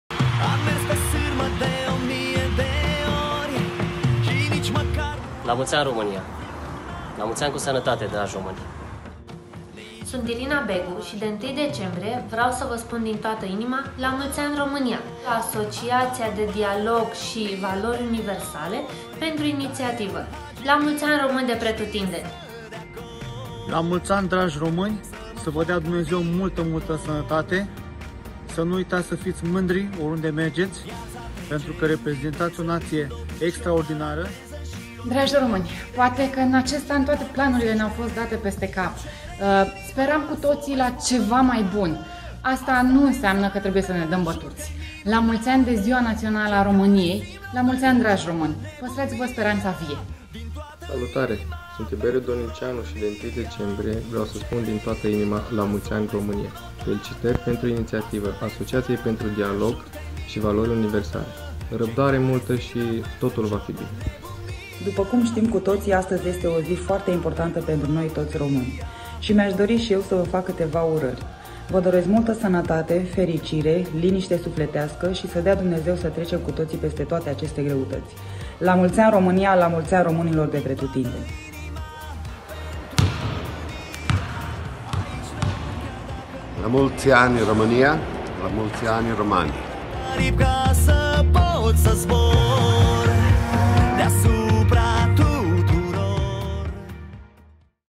ADVU 1 Decembrie 2020
Asociația pentru Dialog și Valori Universale
Va ureaza La mulți ani!

Impreuna cu sportivii campioni ai României.

La mulți ani, Români!
La mulți ani, România!

Uniți prin valorile sportive vom contribui la o societate responsabila

Lucian Sanmartean - Fotbalist
Irina Begu - Jucatoare de tenis
Catalin Morosanu - Kickboxer
Ana Maria Popescu - Scrimea
Tiberiu Dolniceanu - Scrimer
Oana Andreea Manea - Jucatoare de handbal
Nedeljko Lazic - Antrenor de baschet